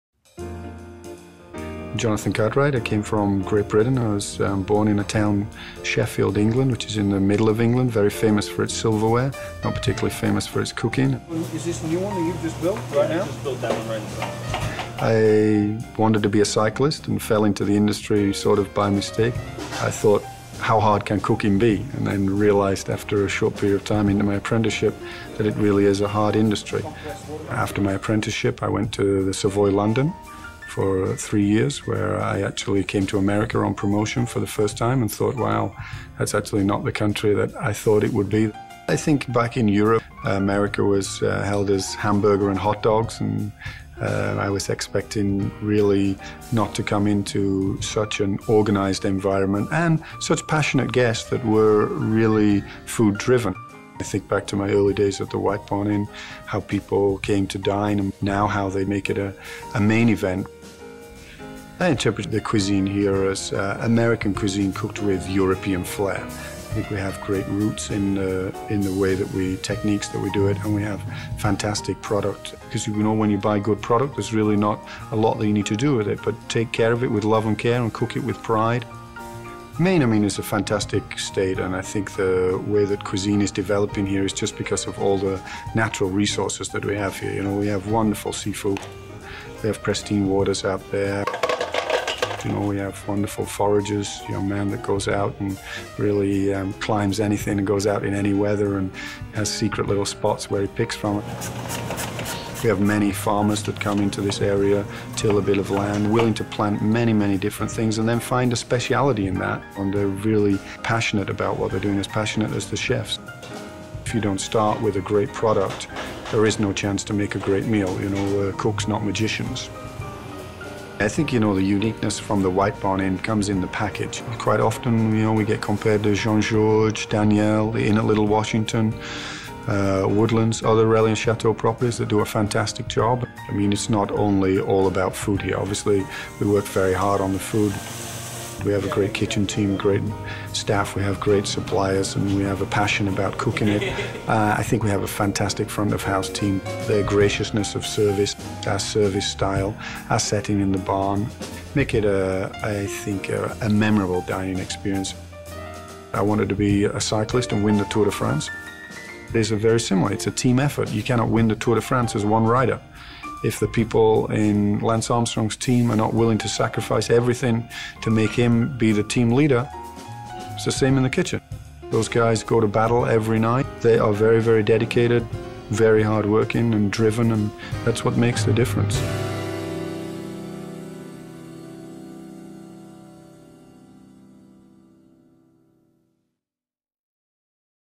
Maine Cuisine - Jonathan Cartwright - The White Barn Inn
A Five Diamond Chef Brings Overseas Flavor to a Maine Kitchen

Jonathan Cartwright never dreamed of being a chef. "I wanted to be the first Englishman to win the Tour de France," says the native of Sheffield, England. He only started cooking after he failed to get a job repairing roads—a position that would have allowed him to cycle on weekends.

But as he tried his hand in various restaurants, including The Savoy in London, Cartwright decided that cooking and cycling have a lot in common. "They required similar teamwork, commitment, dedication, hard work, and patience. I found that I enjoyed cooking very much."
The White Barn Inn Marries Local Ingredients with European Style

He came to Kennebunkport's White Barn Inn in 1996. Now it's the only restaurant in New England to win the AAA Five Diamond and Mobile Five Star Awards.

Set in a renovated 1860s' barn, the ambiance features rustic post-and-beam architecture with elegant accoutrements, tuxedoed waiters, white linens and candlelight. The cuisine is classical gourmet, marrying local ingredients and a European style of cooking, beautifully presented. Cartwright suggests trying the poached lobster on handmade fettuccine with cognac coral butter.